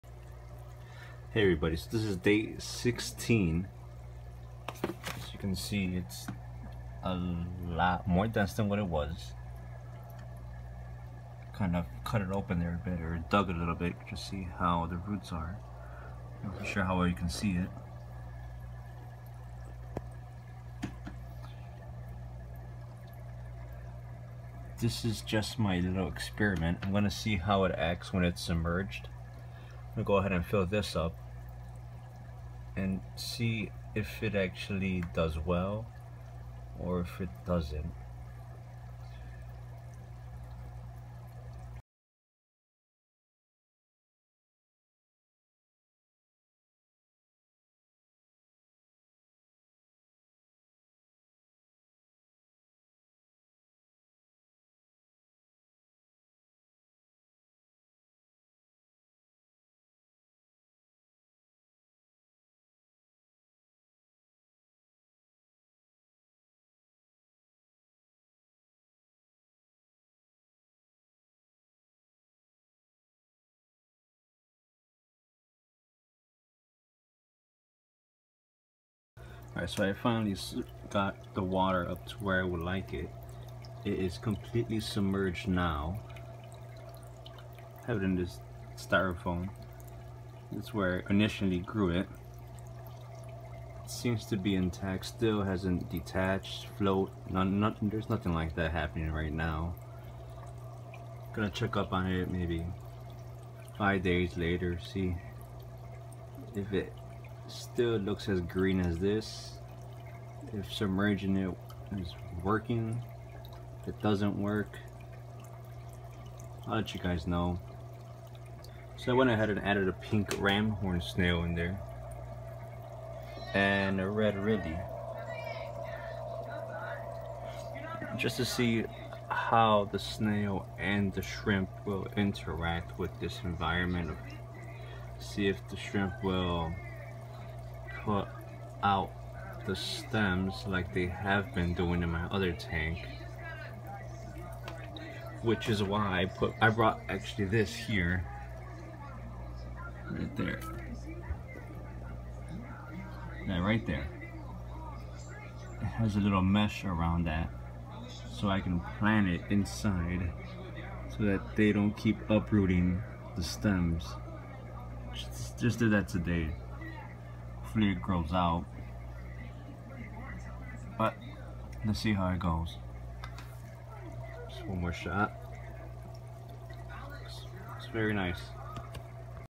My glossostigma experiment
I decided to submerge the glossostigma to see just how well it does, update will be in another 5 days